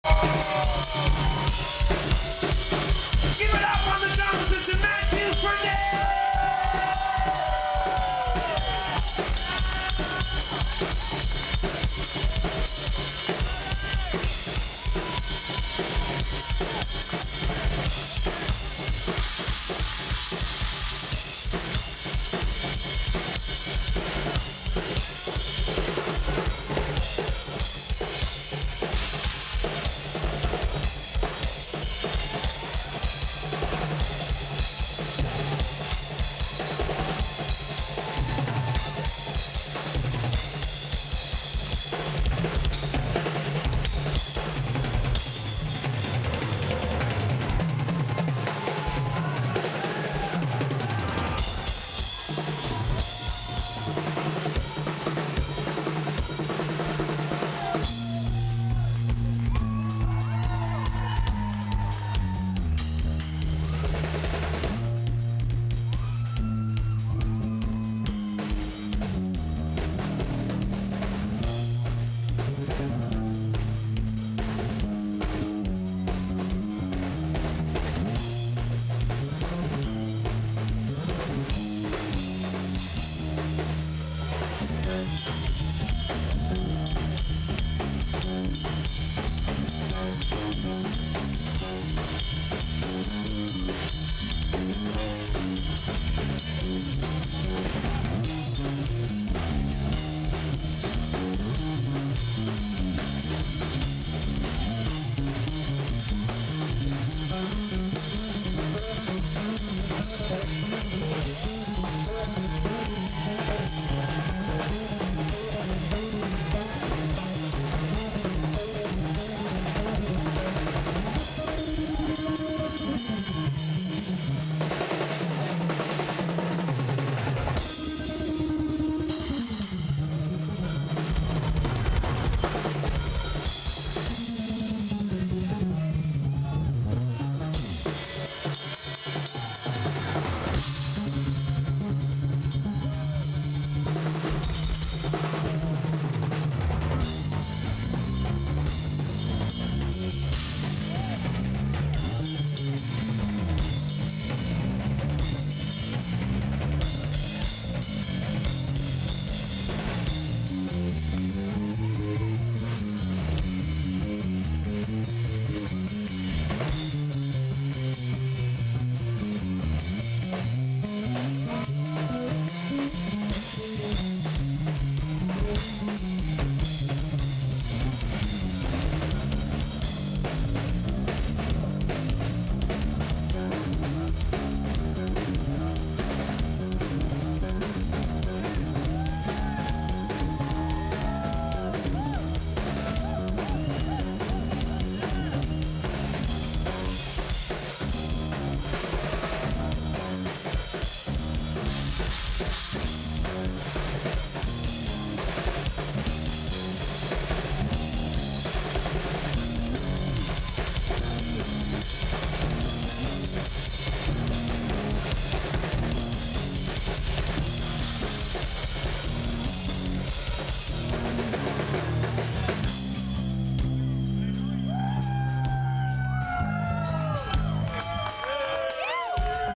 DRUM and BASS SOLO - LOVERBOY - MISSION - 2009 - SHORT VERSION
This Smokin hot Drum and BAss solo is from MAtt Frenette on Drums and Ken Sinnaeve on Bass. This is a Short version of this part of the show in Mission 2009. Matt Frenette the drummer seems to be accually SMOKIN !
The longer version will be uploaded later.
Video is shot by Gene Greenwood of Farting Puppy Productions.